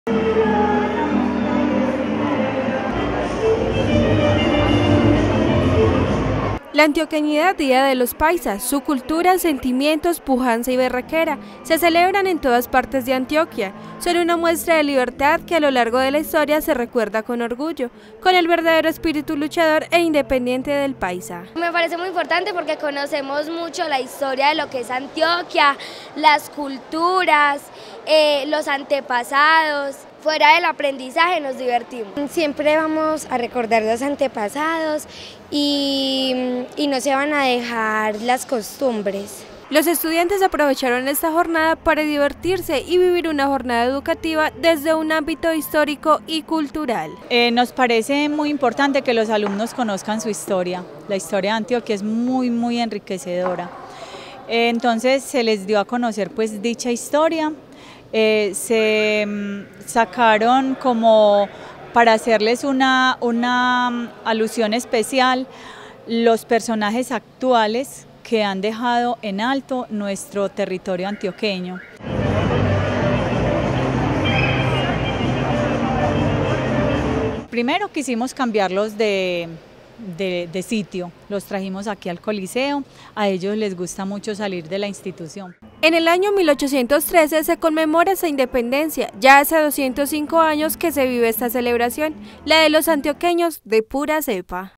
Festejo a lo paisa en la I.E Normal Superior
El día jueves 9 de agosto se organizó por parte de la Institución
Educativa Normal Sagrada Familia la celebración de la Antioqueñidad, un espacio de cultura y tradición